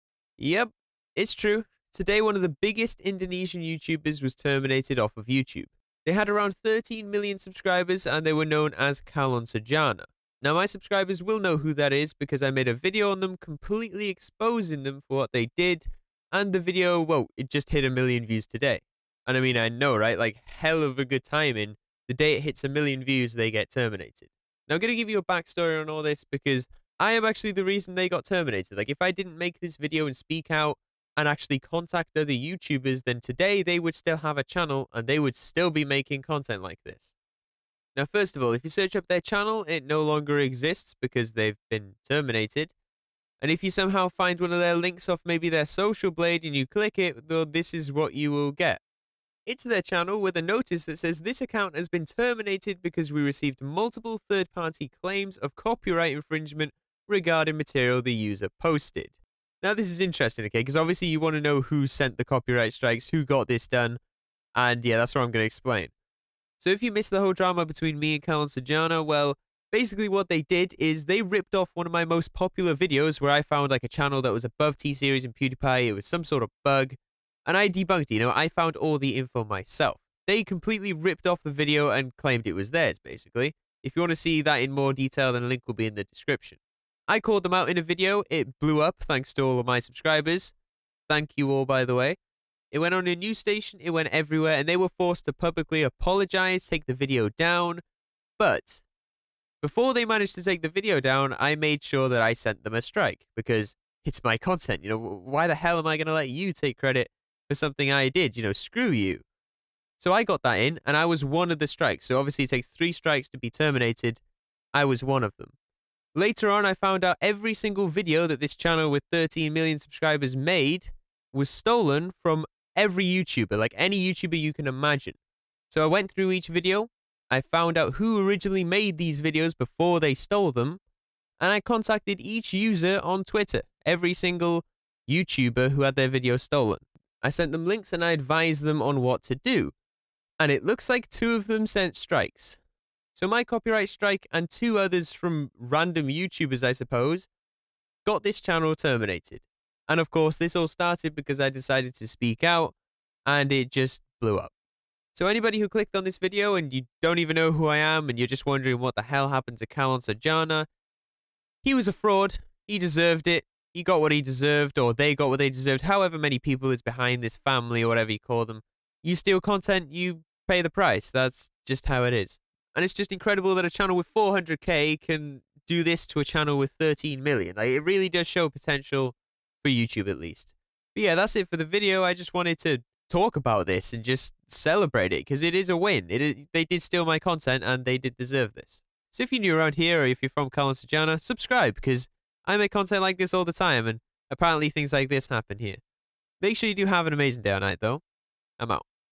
i terminated a YouTuber with 13 million subs...
explaining how i TERMINATED Calon Sarjana... because today their channel was deleted from youtube. One of the biggest Indonesian youtubers on the platform gone.